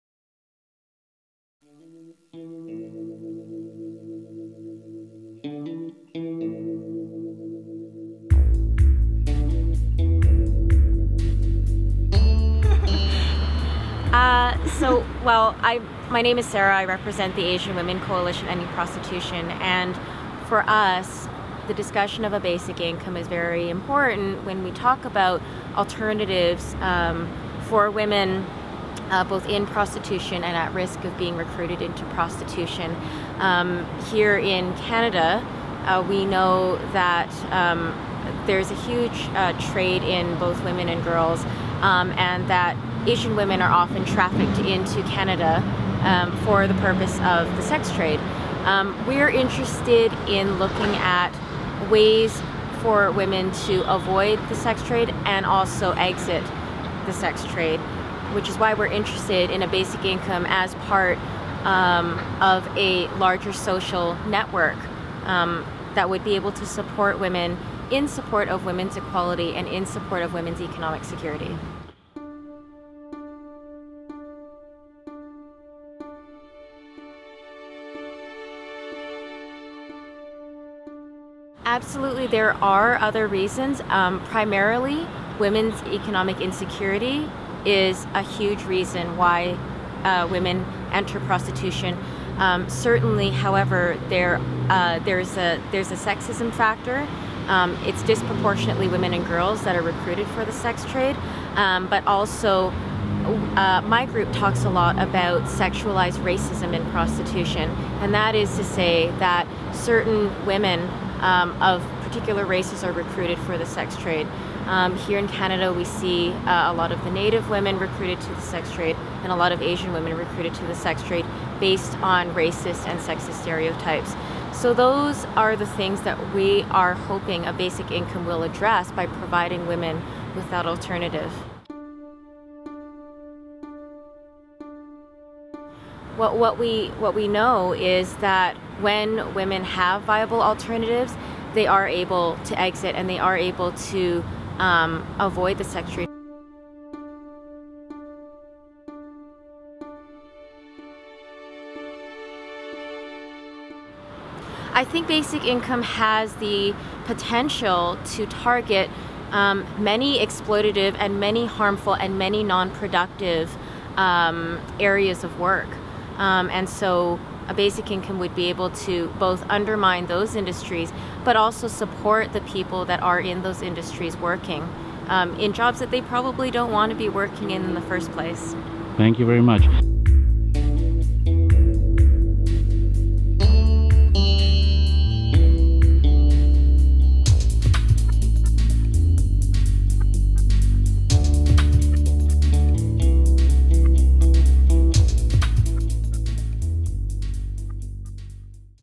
Basic Income and Ending Prostitution
Enno Schmidt interviews Sarah M. Mah from the Canadian “Asian Women Coalition Ending Prostitution“. She talks about the implications of an unconditional basic income on prostitution and on equality.

The video was filmed at the BIEN Congress in Montreal, June 27th 2014.